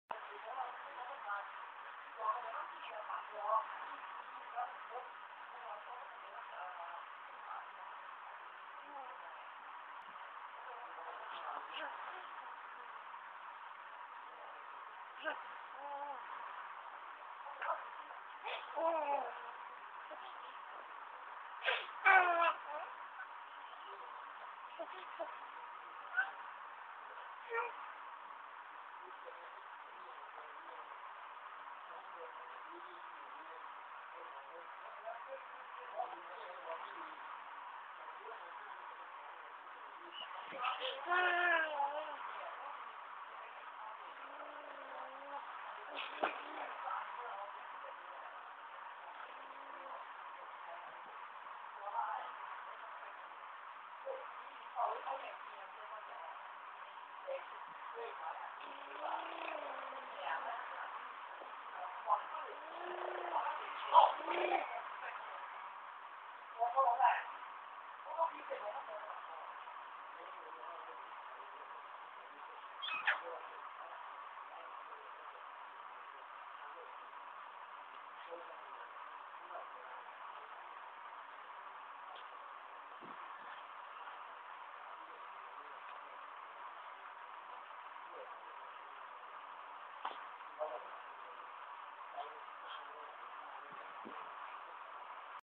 Raikkonen's "talking" pillow
this is cute.. Raikk saw this greeny cartoon pillow & we first found out when i was lying beside him.. he crawled twds this pillow, assuming that the cartoon is "something talkable" which seems replacing "sua ku".. whahaha

he blabbers his baby language, smile, drool & laughed to this pillow, and when he helped the pillow right up, i pushed it down, he started his cute laughters & screamings!! horrible boy!!

well, in kid's world, they're Naive BUT naive in their innocent ways.. i Love You, Raikko!!